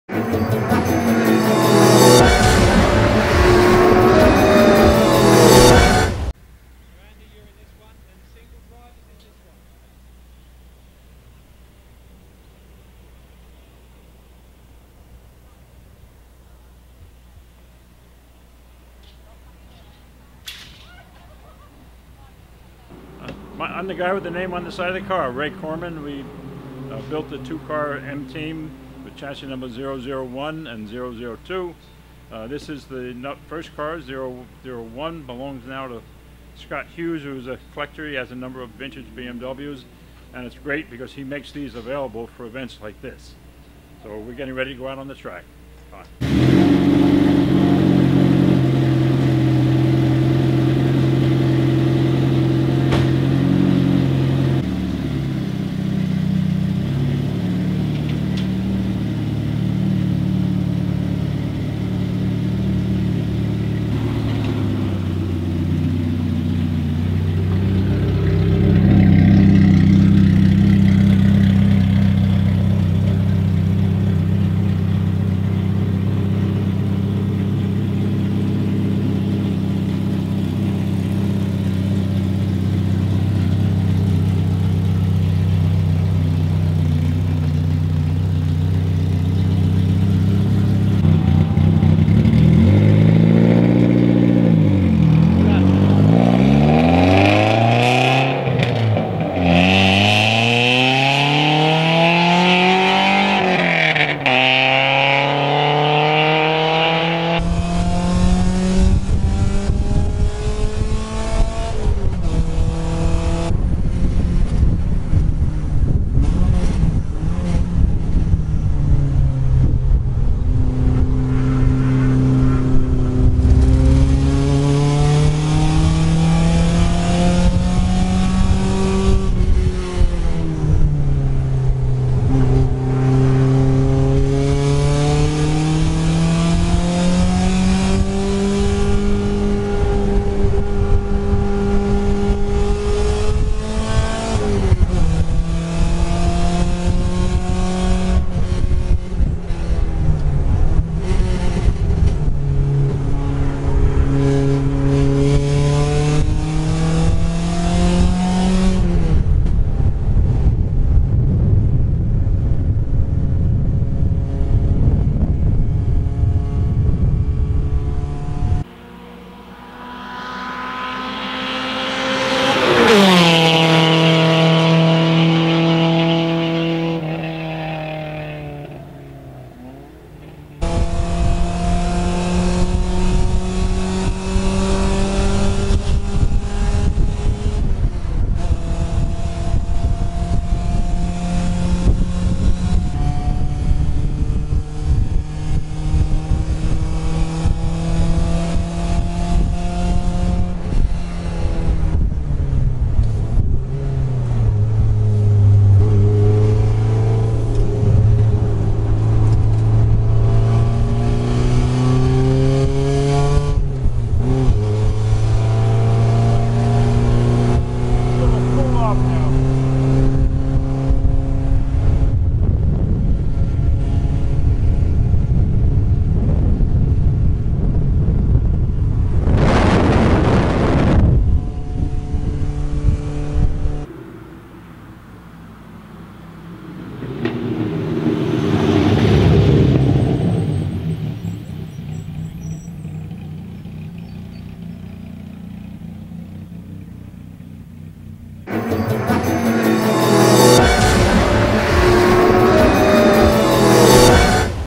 BMW M3 001 at Road America driven by Roy Korman
In 1987, under contract with BMW North America, Korman raced a pair of BMW M3 Sport Sedans, chassis numbers 001 and 002.  In their first season the "M-Team" scored a historic victory in the IMSA Watkins Glen 500 Mile Race, the first time in the four year history
of the Series that a team finished one-two, also setting a new qualifying lap record and fastest race lap record.  Korman Autoworks
prepared BMW's have scored 26 top three finishes in the IMSA Firehawk Endurance Series, including two 24 hour race victories.